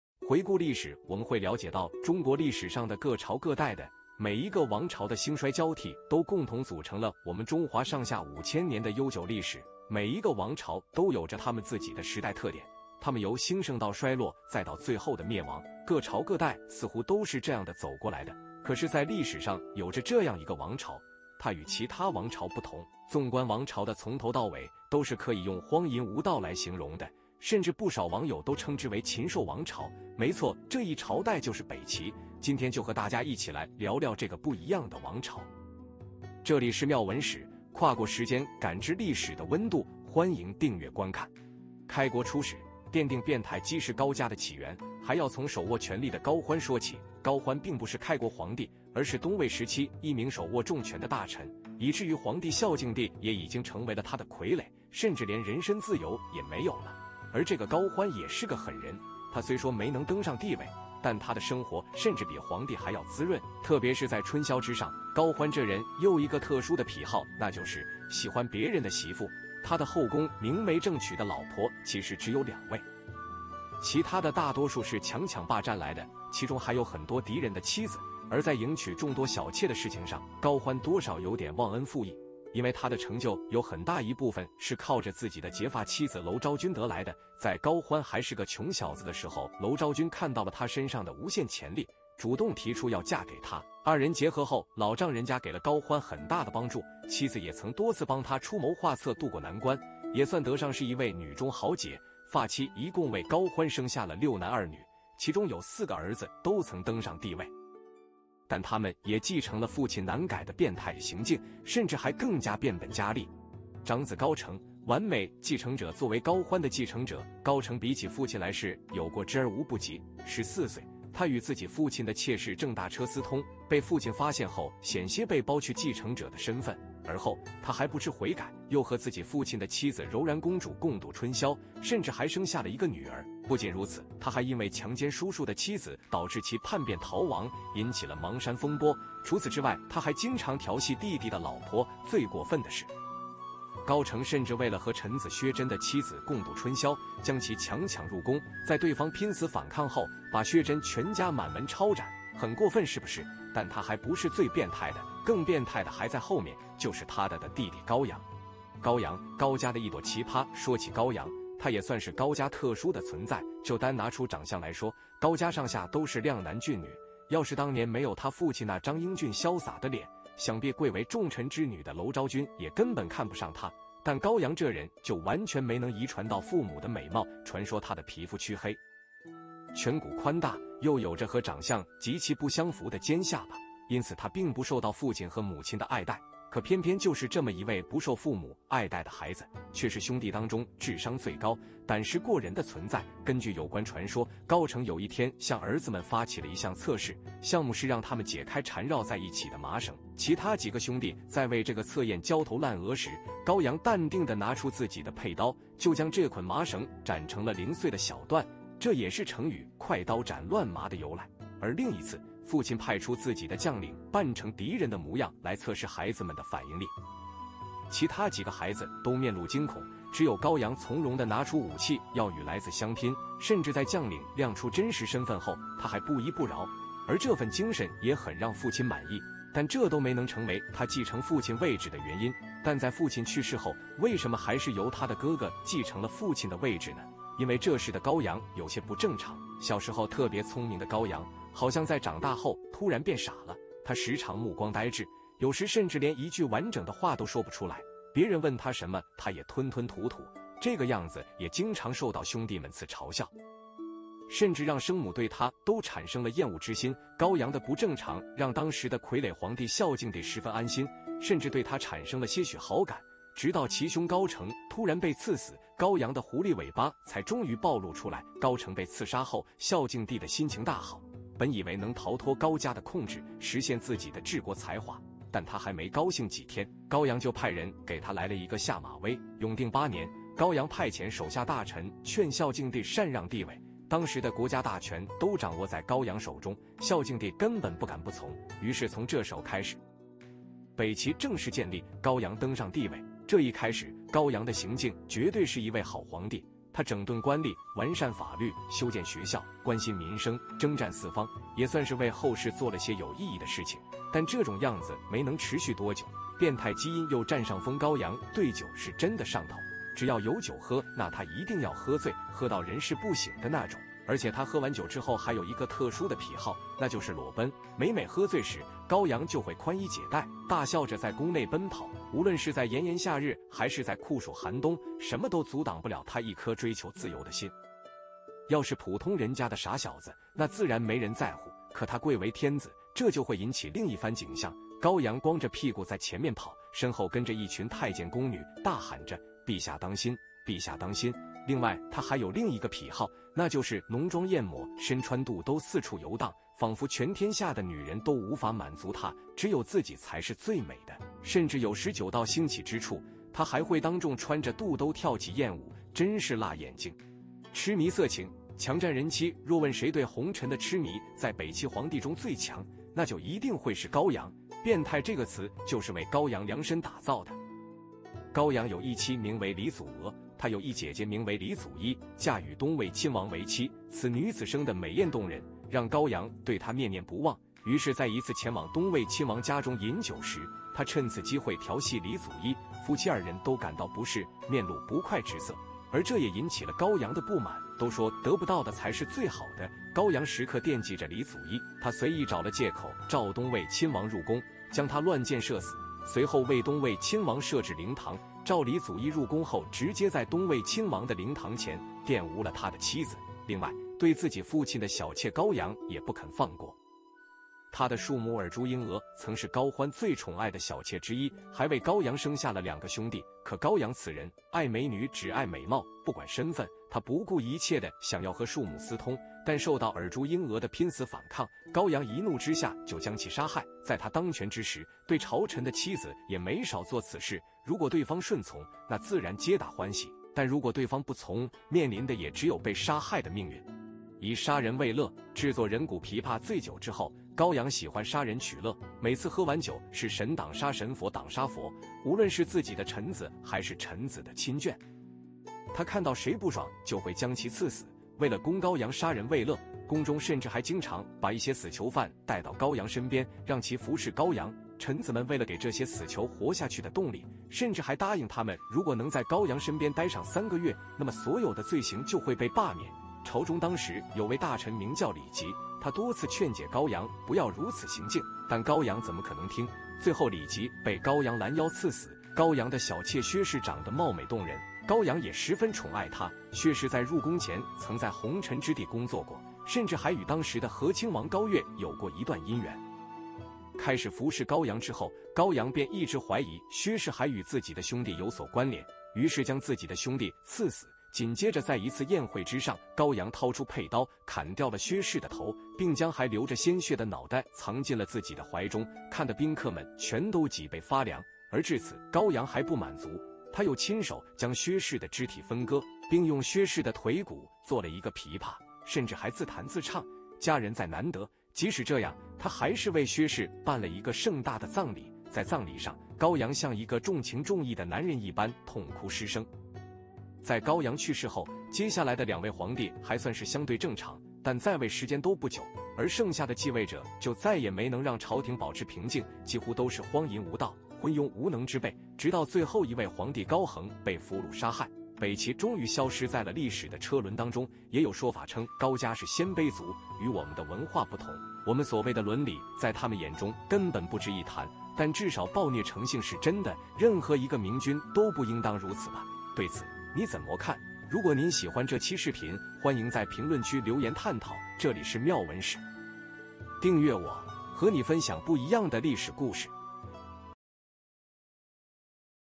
歷史上存活了28年的北齊王朝，被稱為「禽獸王朝」，荒唐奇葩程度根本難以想象！【妙文史】歷史
回顧歷史我們會了解到中國歷史上的各朝各代的，每一個王朝的興衰交替都共同組成了我們中華上下五千年的悠久歷史。每一個王朝都有著它們自己的時代特點，它們由興盛到衰落再到最後的滅亡，各朝各代似乎都是這樣的走過來的。可是在歷史上有著這樣一個王朝，它與其他王朝不同。縱觀王朝的從頭到尾，都是可以用荒淫無道來形容的，甚至不少網友都稱之為「禽獸王朝」，沒錯這一朝代就是北齊。今天就和大家一起來聊聊這個「不一樣的」王朝。歷史 歷史故事 歷史人物 古代歷史 揭秘 請注意，本影片是專注於講解社會新聞和奇聞異事，完全符合YouTube社群準則。